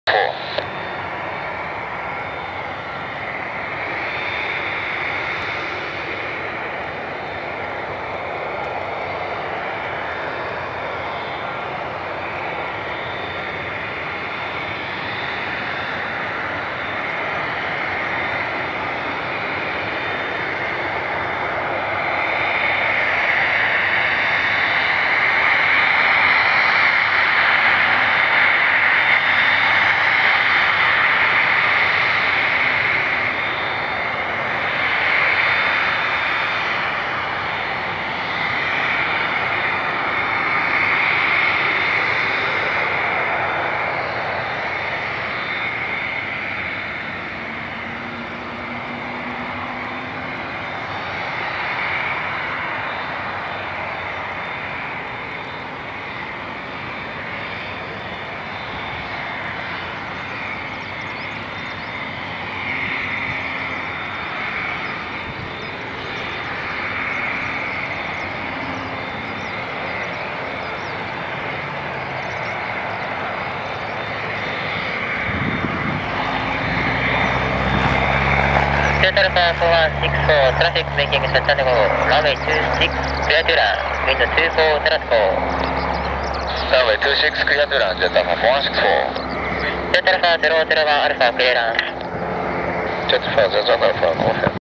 JA4040 [Osaka Aviation] Cessna 172P Skyhawk II TGL RWY26 RJOT
Feb.20th-2018
@TAK/RJOT Japan
高松空港
大阪航空